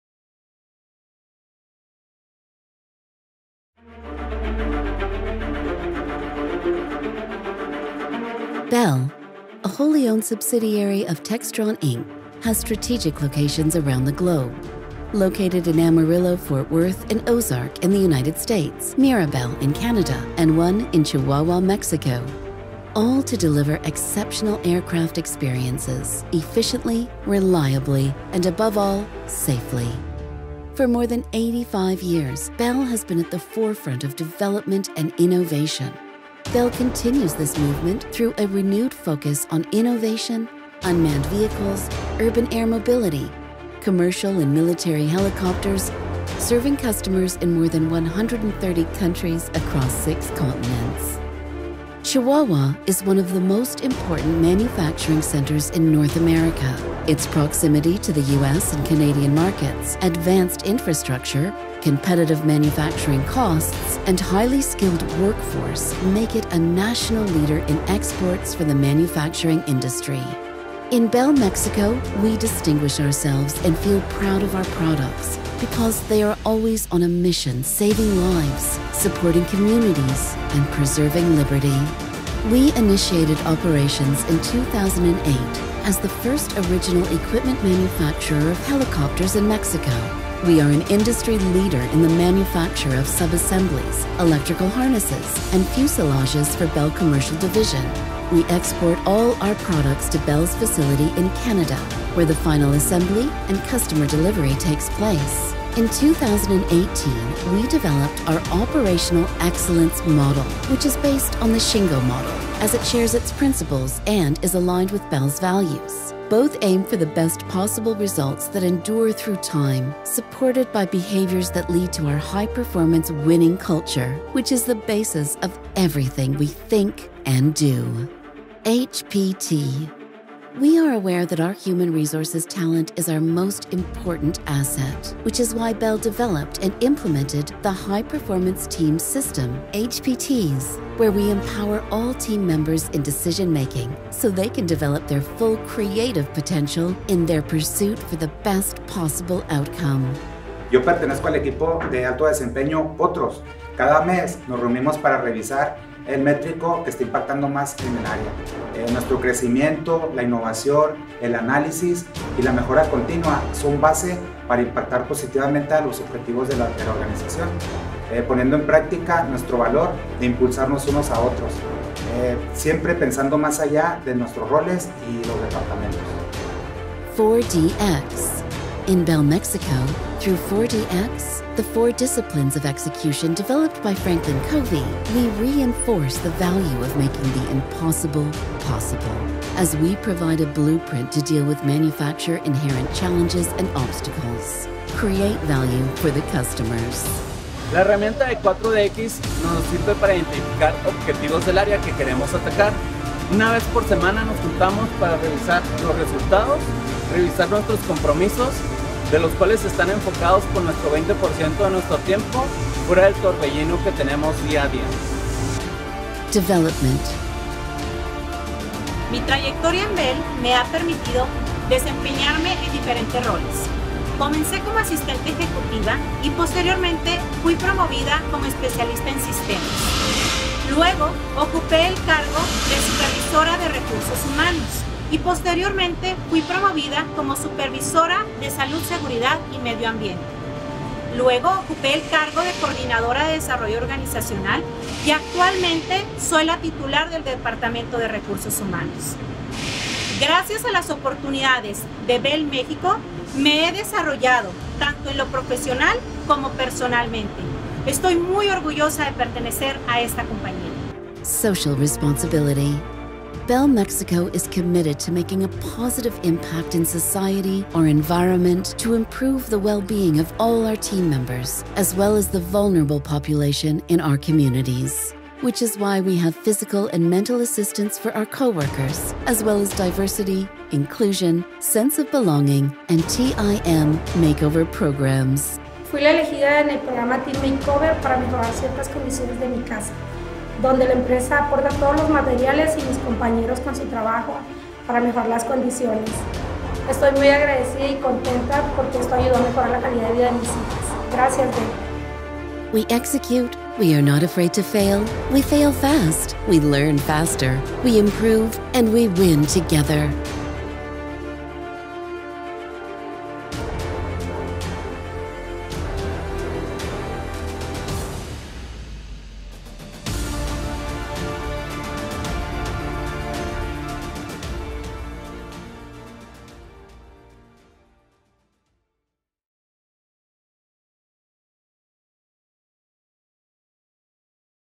Bell | Shingo Silver Medallion Recipient | 2022
Shingo Silver Medallion awarded to Bell in Chihuahua, Mexico for organizational excellence.